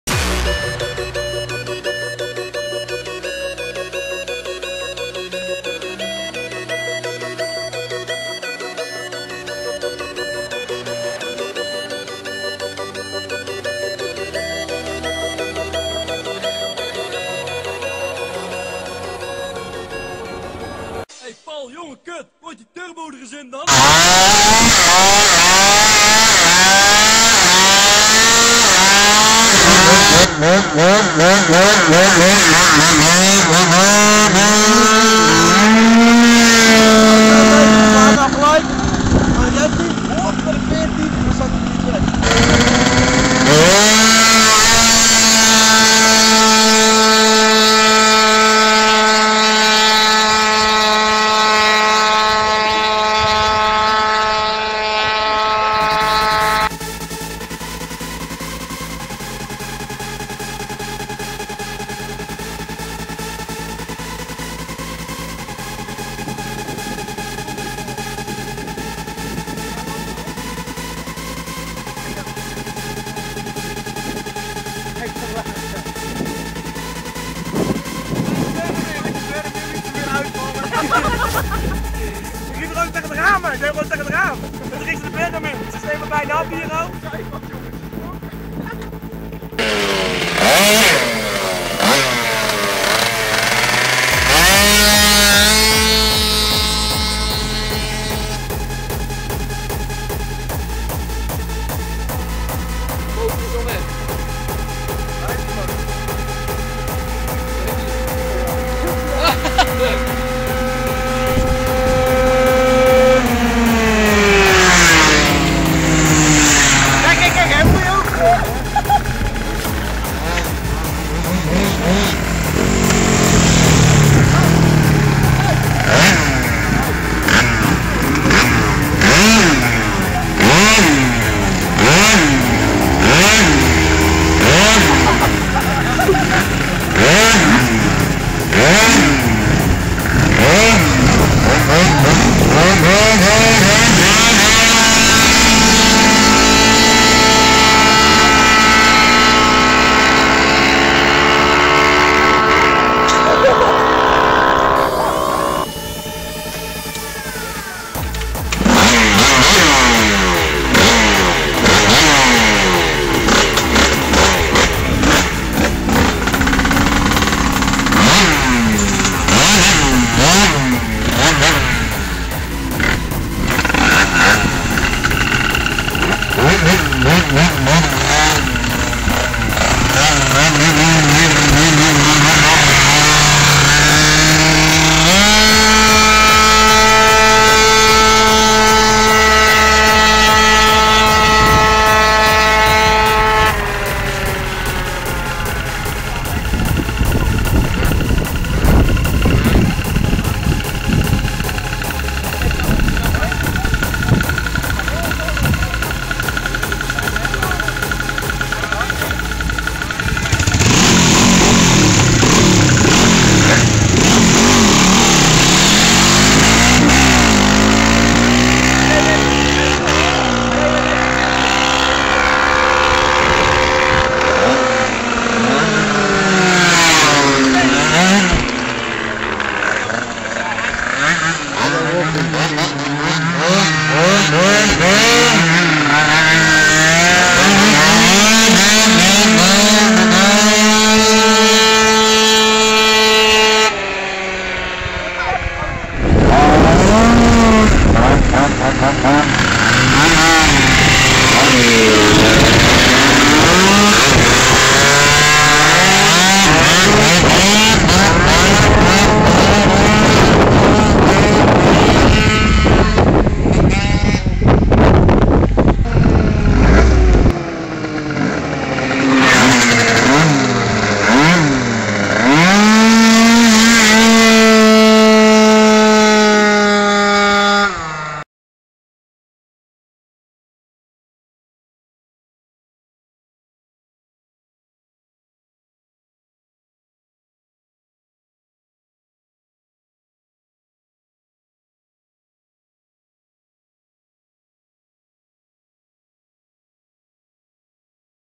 Sprint scooters Beverwijk corusweg ABC Scootercentrum Heemskerk
Sprintscooter op de corus weg in beverwijk, cowboy billy kwam ook nog ff langs op ze motor .. geen idee wie die kerel was. maar na het racen tegen ons wist ie wel wie wij waren.